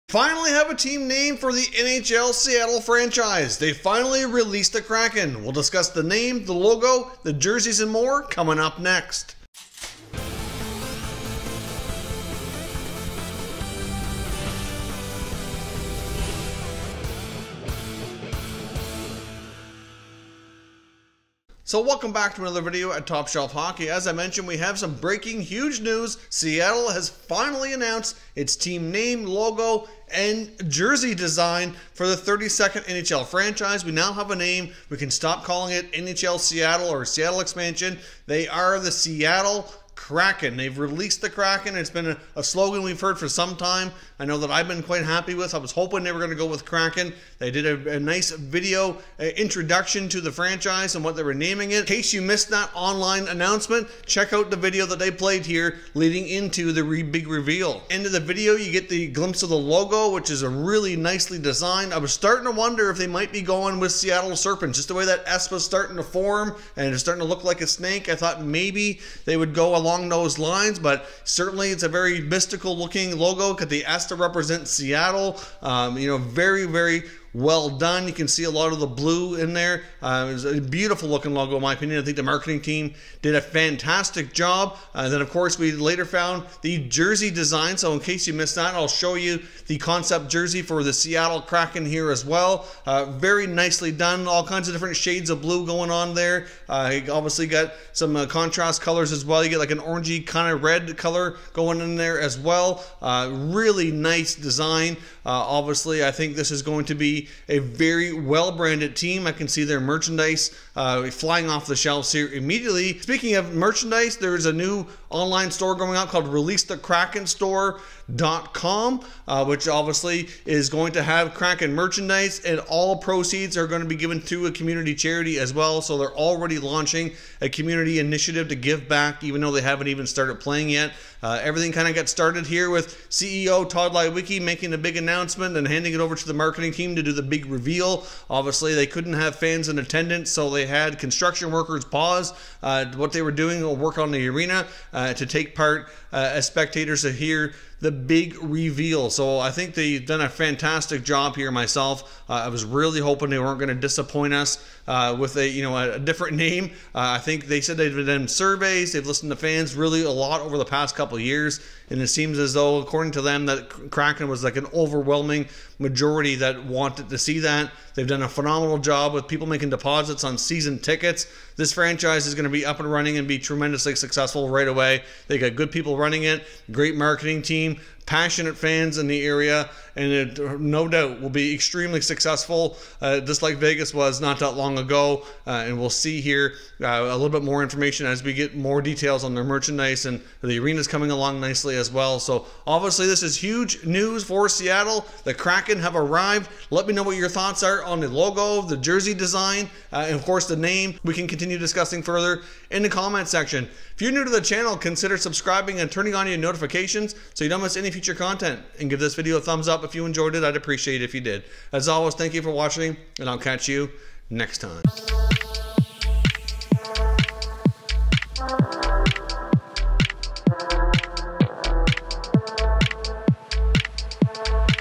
NHL Seattle Kraken Revealed (Name, Logo & Jersey) - NHL Breaking News
Breaking News: Seattle Kraken Revealed (Name, Logo & Jersey)
NHL Seattle Name, NHL Seattle Logo and NHL Seattle Jersey all revealed today.

Keep Watching Top Shelf Hockey:

Track Info: MusicSports